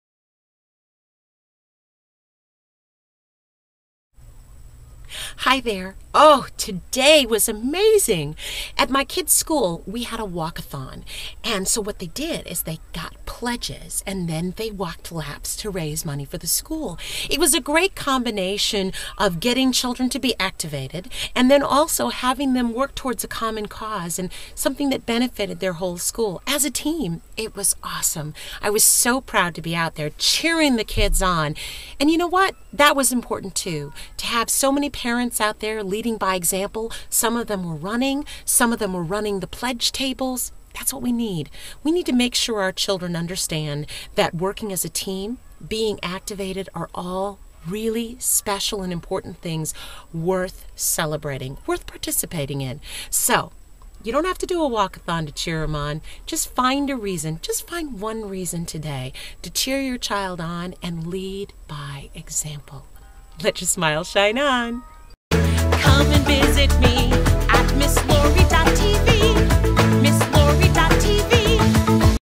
Miss Lori says "Cheer your child on and lead by example!" video
Miss Lori is someone that kids have fun with and parents trust. Learn more about all that Miss Lori does for the community by logging onto her performance and her blog, "Musings from my Minivan,"